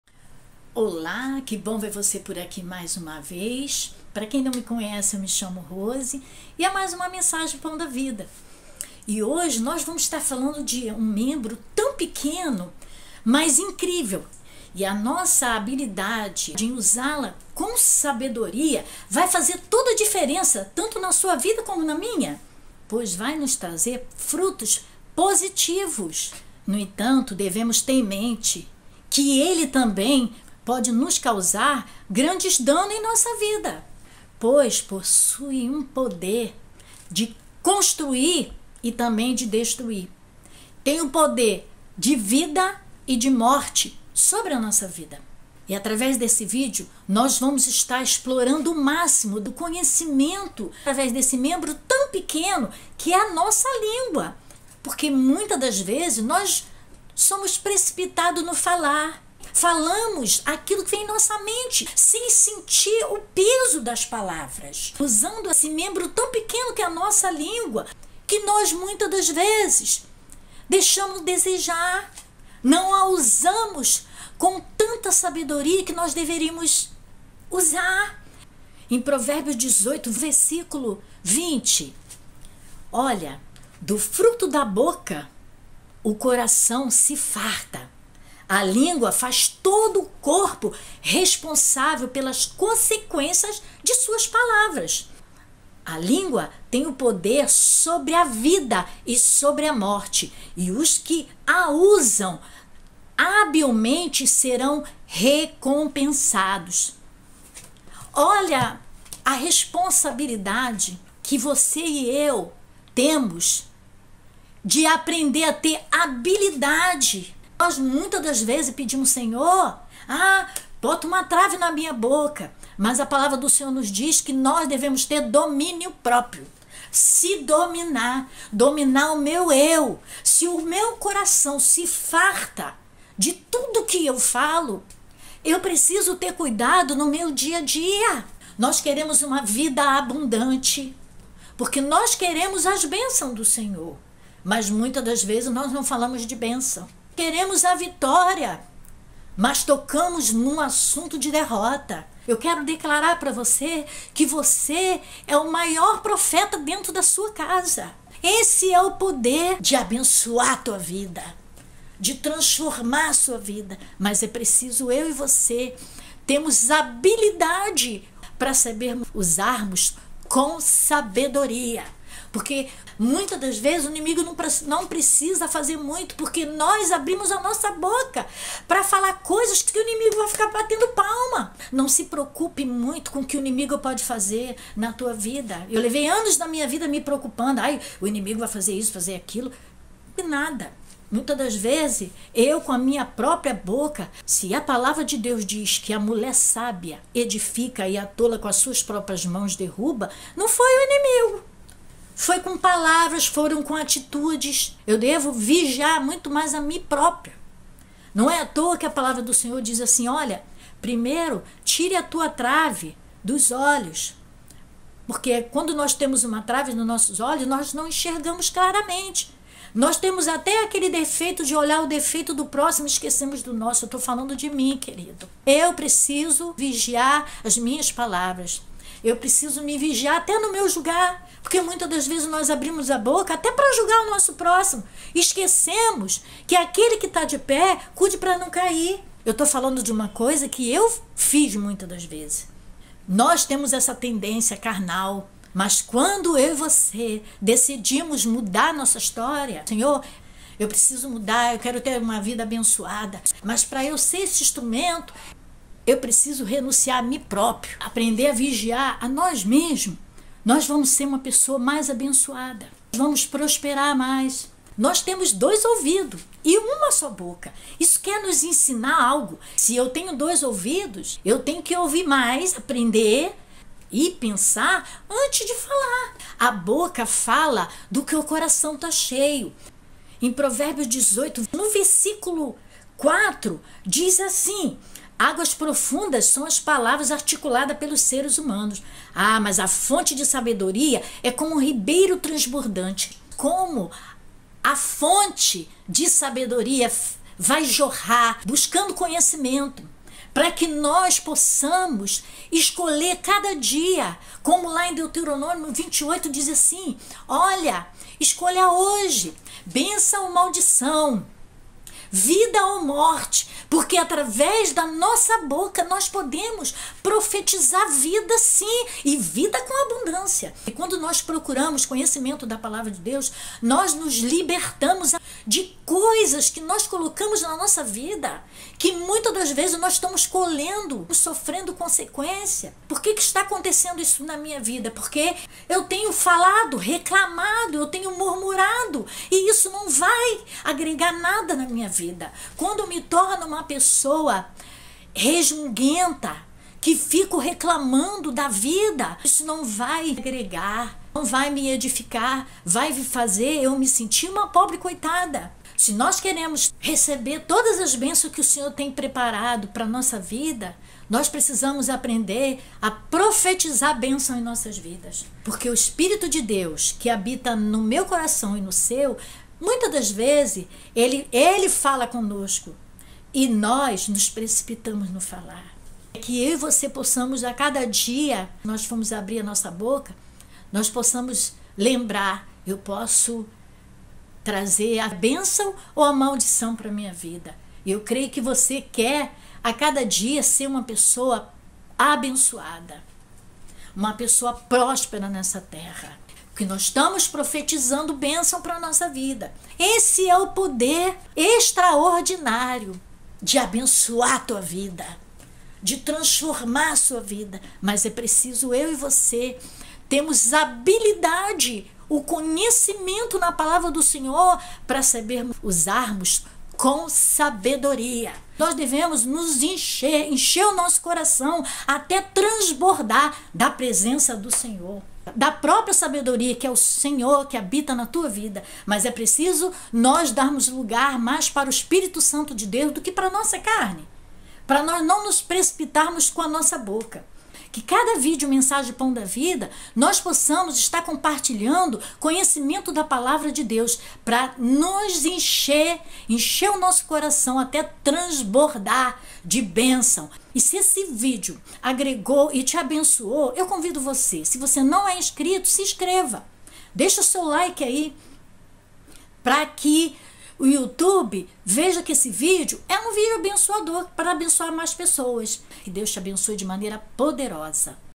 "O GRANDE PODER da LÍNGUA"
Desvendando o grande poder da língua quando aprendemos a ter habilidade com um órgão tão pequeno que tem grande influência em nossas vidas porquê muita das vezes o inimigo não precisa fazer muito nós com nossa própria boca falamos palavras de maldição sem pensar nas consequências. Você vai entender que um membro tão pequeno mas incrível que é a nossa língua que exerce uma grande influência em sua vida e até em sua saúde pois tem o poder de construir e de destruir poder de vida e de morte e preciso ter habilidade pra saber usa com sabedoria pois tem muito poder em provérbios 18 v 20 do fruto da boca o coração se farta a língua faz todo o corpo responsável pelas consequências de suas palavras e quem usá-la habilmente serão recompensados isso vai fazer toda diferença porque muita das vezes queremos as bençãos mas falamos de palavras de maldição queremos vitória e falamos de derrota a boca falar do que o coração tá cheio e o nosso coração é o depósito de coisas boas ou ruim você tem o poder de escolher.